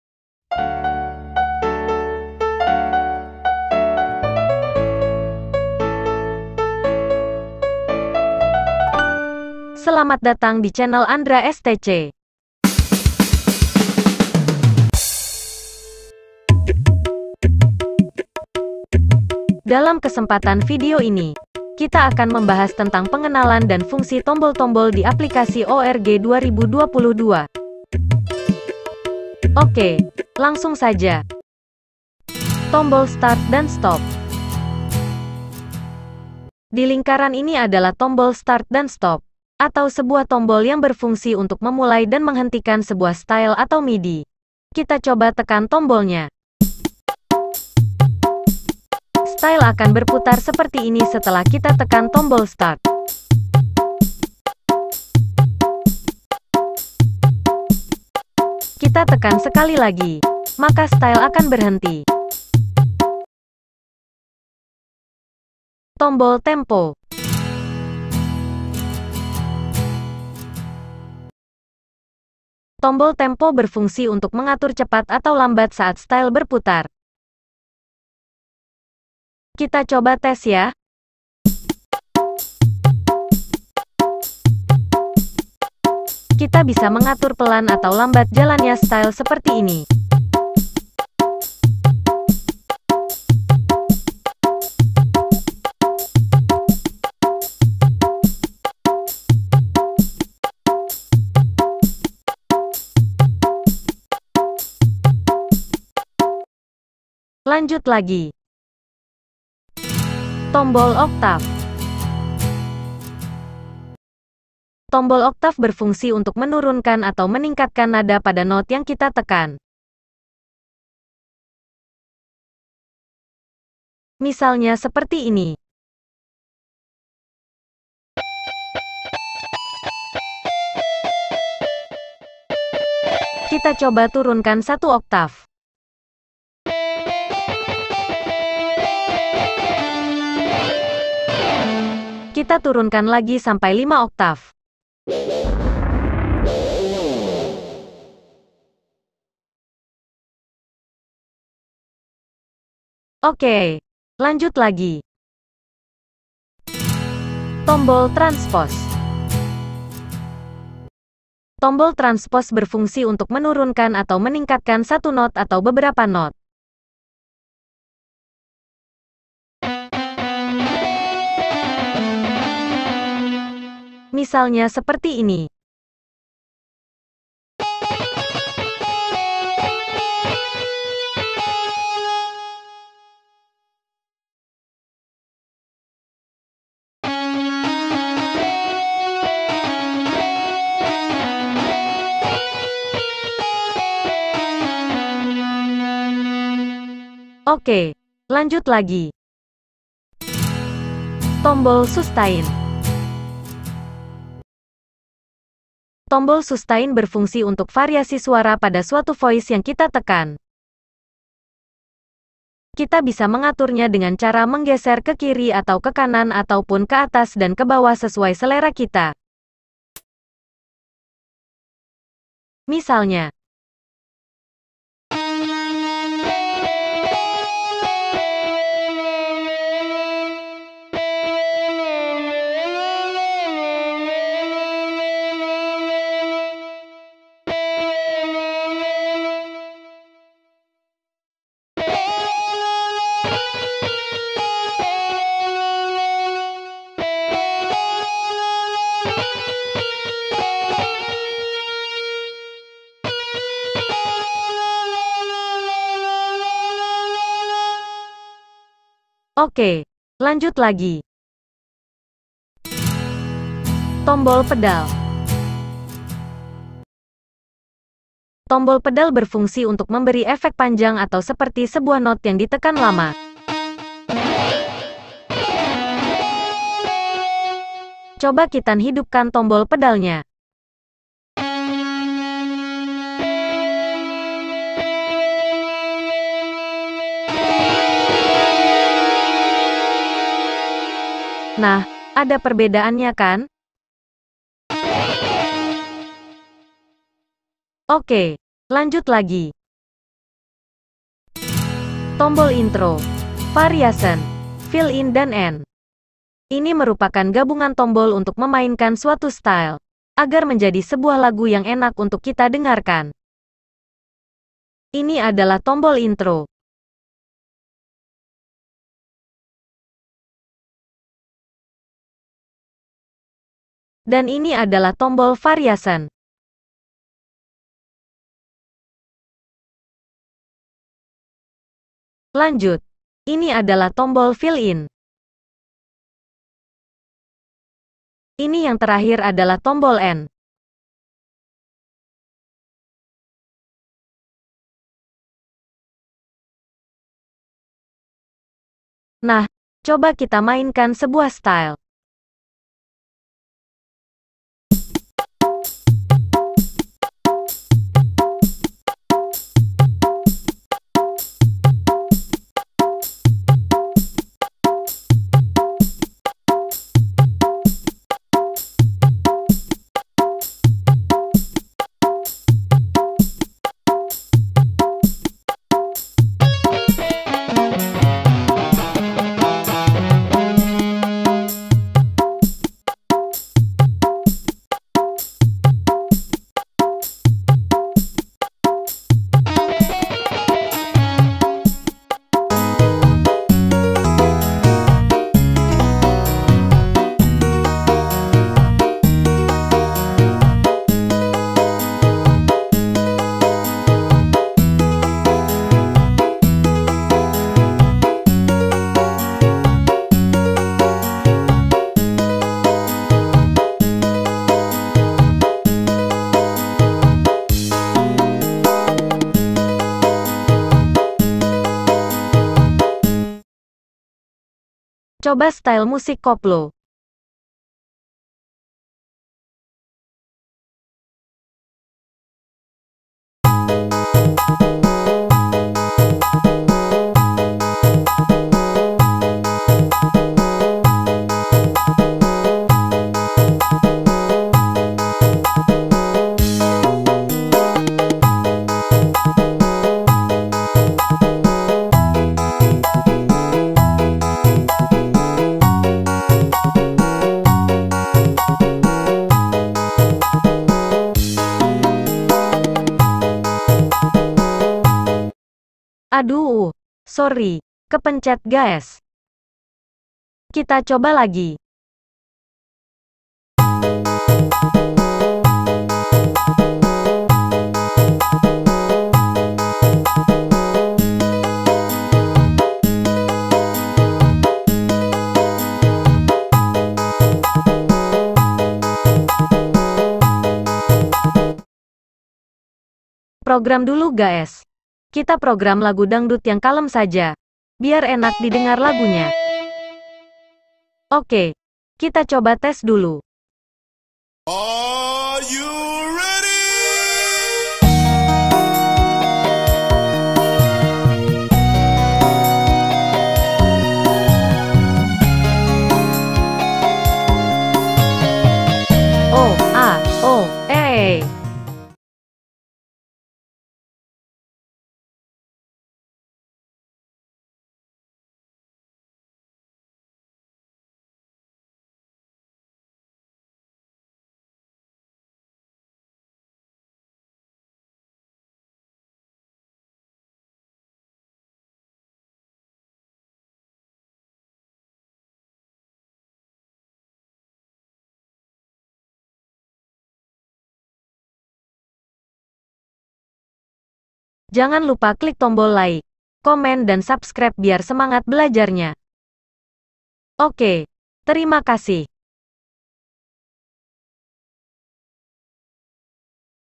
Testing Tutorial Tombol di ORG 2022
Dalam video ini kita akan membahas tentang fungsi tombol di Aplikasi ORG 2022...
Banyak sekali tombol-tombol di Aplikasi ORG 2022, namun kita akan membahas tombol yang sering digunakan saja...
Selamat menonton.